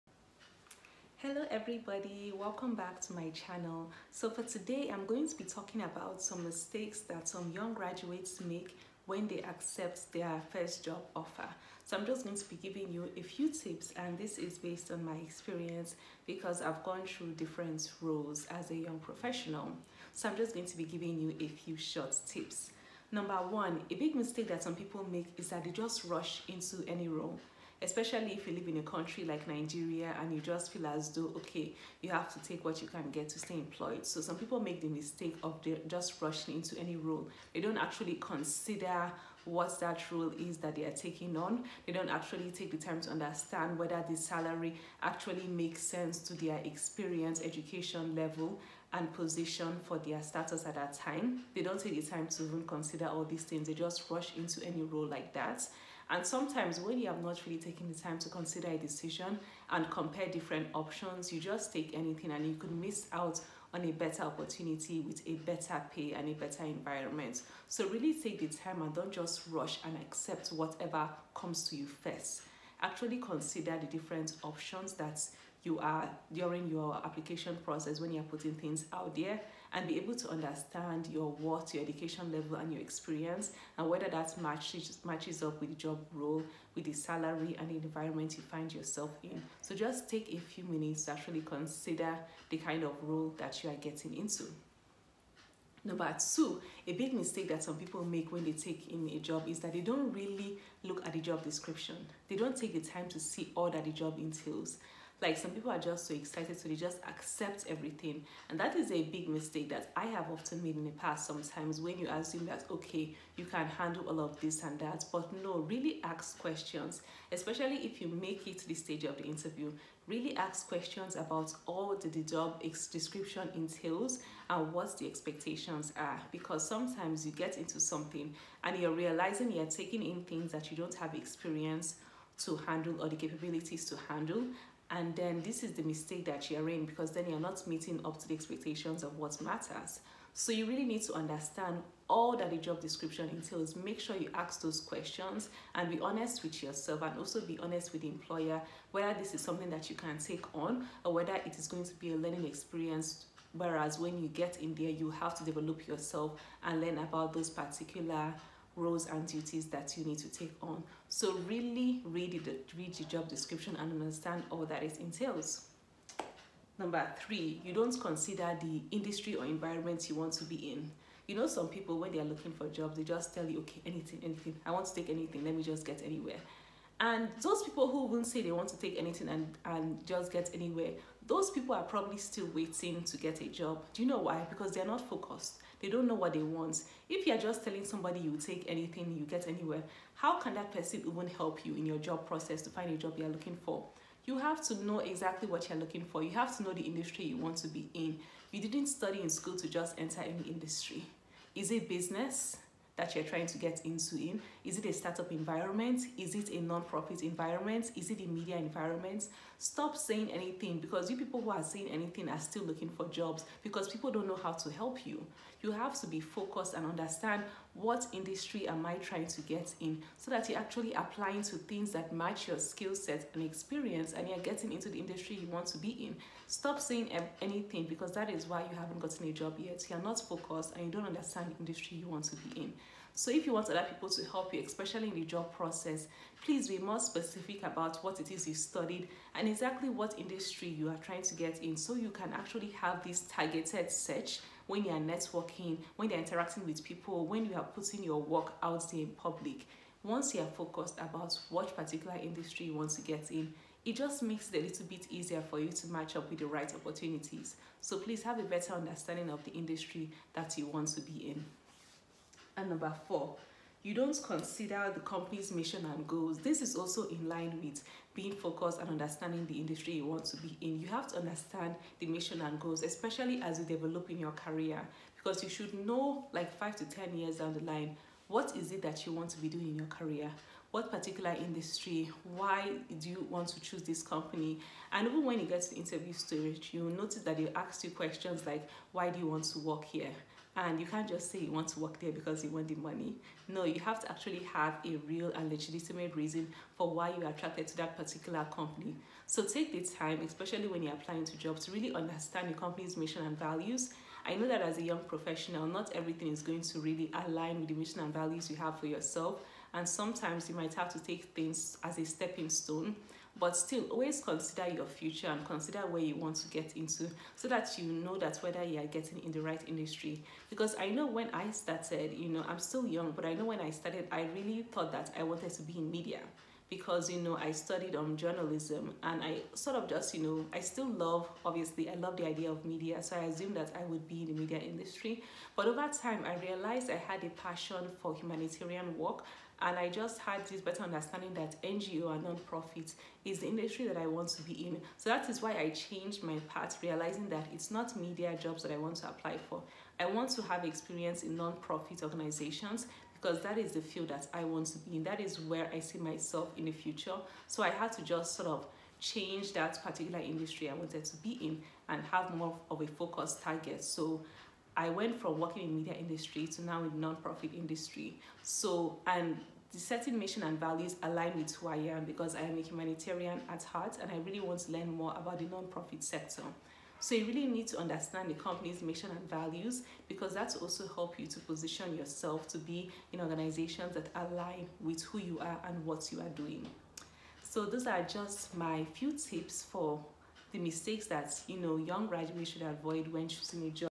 Mistakes To Avoid When Applying for Jobs
After leaving the premise of the academic world, most young professionals are excited and anxious to enter the work world. However, on their quest to get a job, they often make some mistakes. Here are a few mistakes to avoid during your job application process.